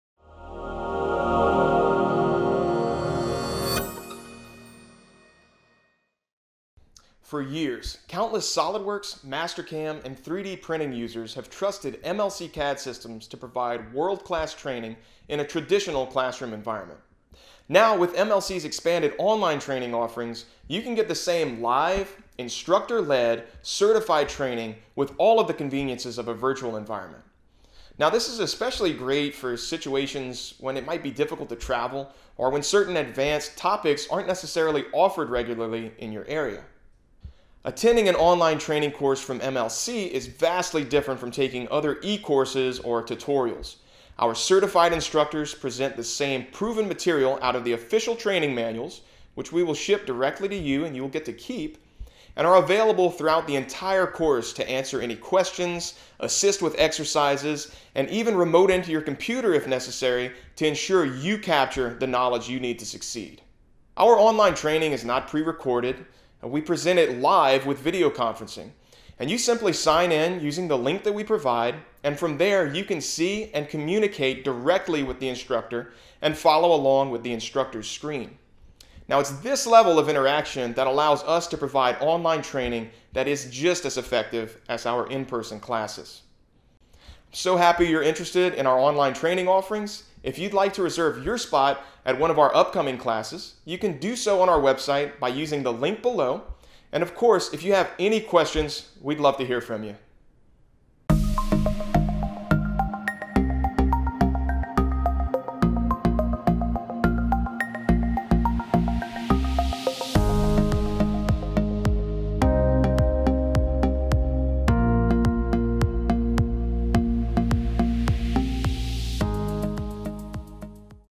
Online Training | Benefits and Overview
MLC CAD Systems provides SOLIDWORKS and Mastercam training from certified instructors in both in-person and online formats. Online training is hosted by a live instructor and the class size is limited to ensure plenty of time is available for interaction and discussion. If you can't find the topic you want at your local office on a date that fits your needs, consider taking the course online! You get the same course material, including a printed manual to use during class and to take notes. You'll also receive the same electronic certificate of completion.